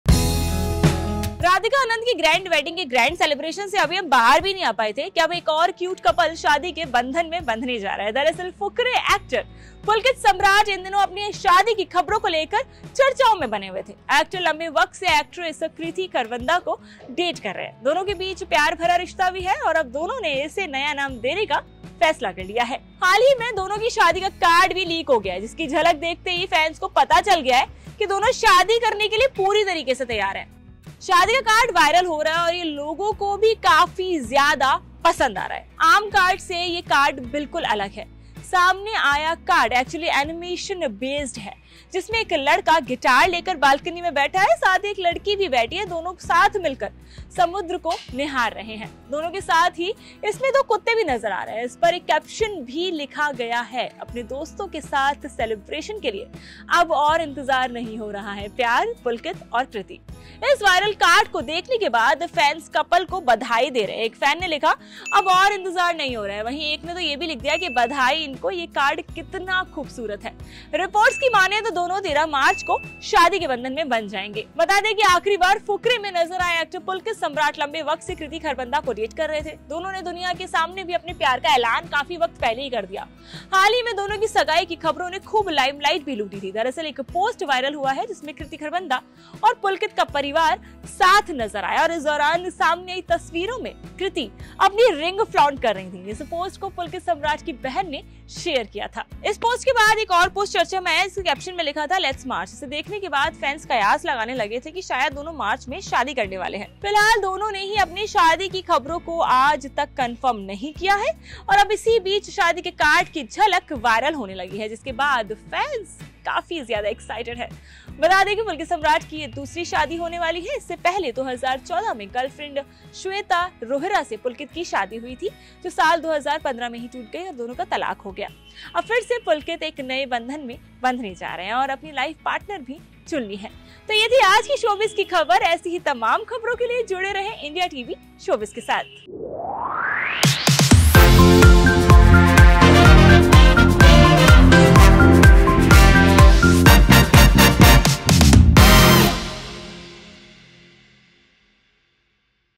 Pulkit Samrat Kriti Kharbanda Wedding Card Leak: पुलकित सम्राट और कृति खरबंदा जल्द लेंगे सात फेरे?
'Fukrey' Actor Pulkit Samrat और Actress Kriti Kharbanda शादी की तैयारी में हैं। दोनों एक साथ जल्द सात फेरे लेने वाले हैं। दोनों की Wedding Card भी अब Leak हो गया है और Social media पर तेजी से Viral हो रहा है।